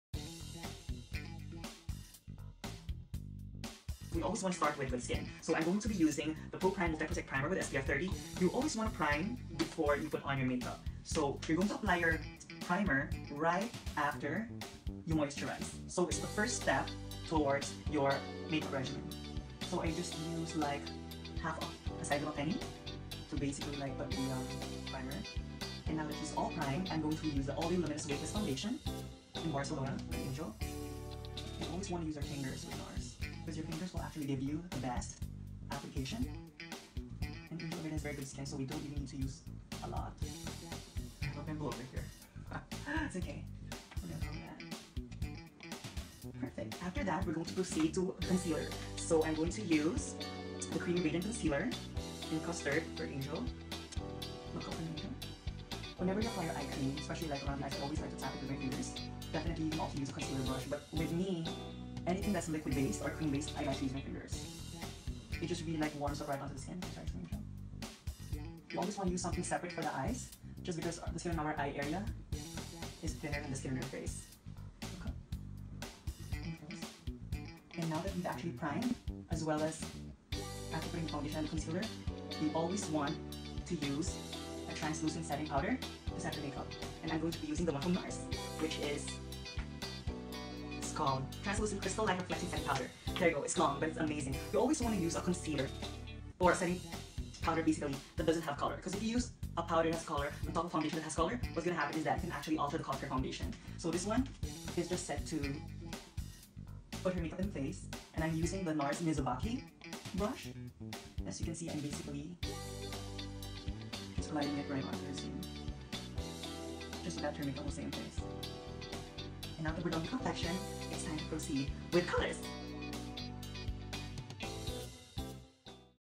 MakeUp Complexion Perfection in 4 Easy Steps by Ally
Sisters Like Us presents Make Up Complexion Perfection in Four Easy Steps by Ally. She applies Nars makeup to her model sister Angel's skin tone. Originally part of the Fall 2016 MakeUp Tutorial, this version contains more detailed tips in the first steps of applying makeup before the colored makeup. This will be an easy reference for our future makeup tutorials.

Featured Nars favorites:
1. Multi-Protect Primer with SPF 30 Sunscreen
2. All Day Luminous Weightless Foundation in Barcelona
3. Radiant Creamy Concealer in Custard
4. Translucent Crystal Light Reflecting Setting Powder

Credits:
Editing and Direction: Angel Qinan
Content and Logistics: Ally Castillo
Music and Editing App: Cute CUT

Thank you for all the love and support of our family and friends!